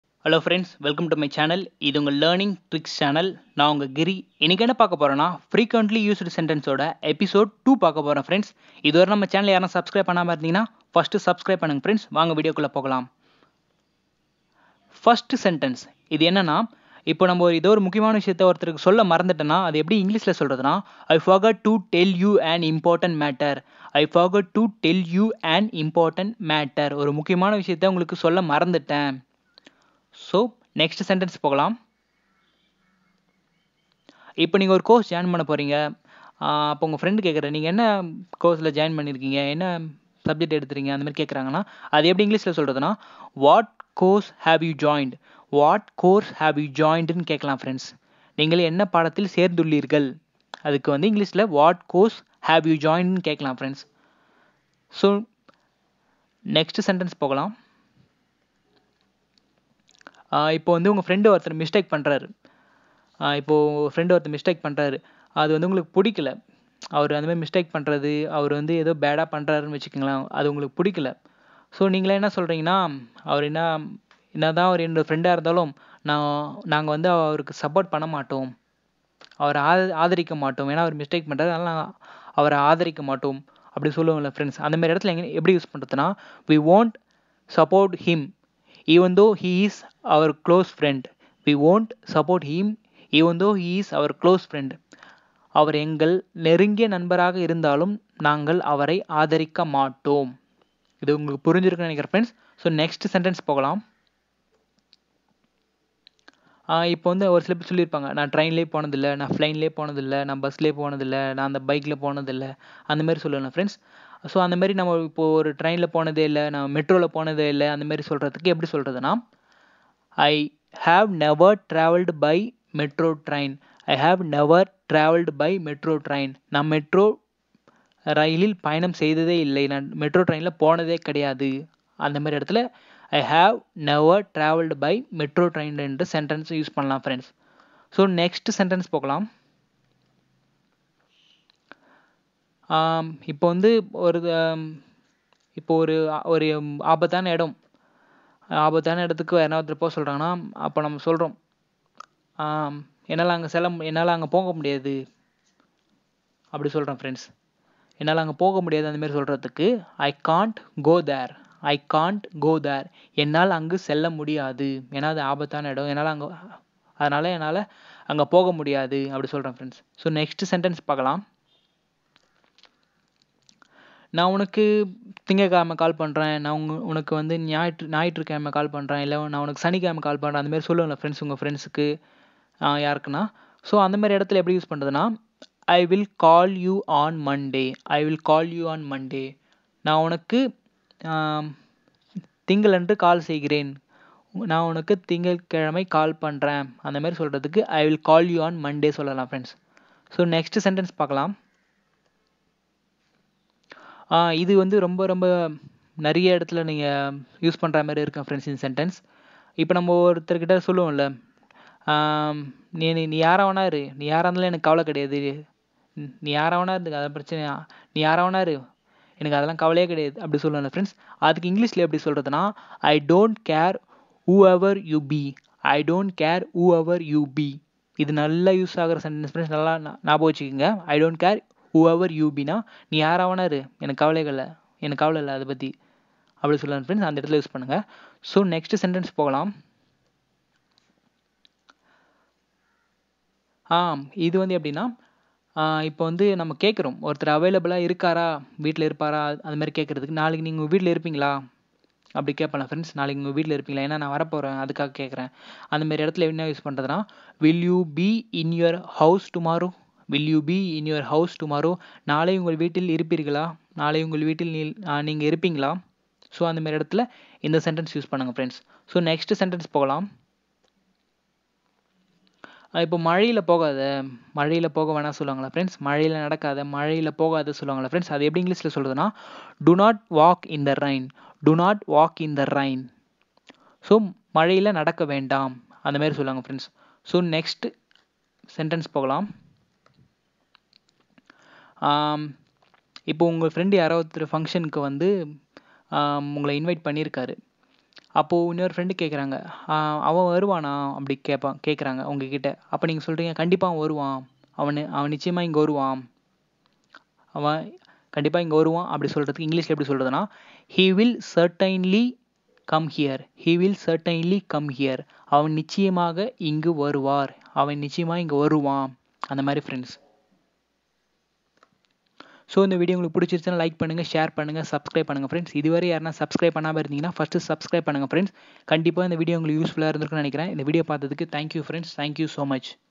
Frequently Used Sentences #2 | Useful Sentences | Spoken English to Tamil | Learning Tricks
In this video, I have explained about Useful Sentences with Tamil meaning.

This Video contains Important Sentences with simple tamil Explanations.

The Sentences are

I forgot to tell you an important matter.

What course have you joined?

We won't support him even though he is our close friend.

I have never travelled by metro train.

I can't go there.

I will call you on Monday.

I don't care whoever you be?

Will you be in your house tomorrow?

Do not walk in the rain.

He will certainly come here.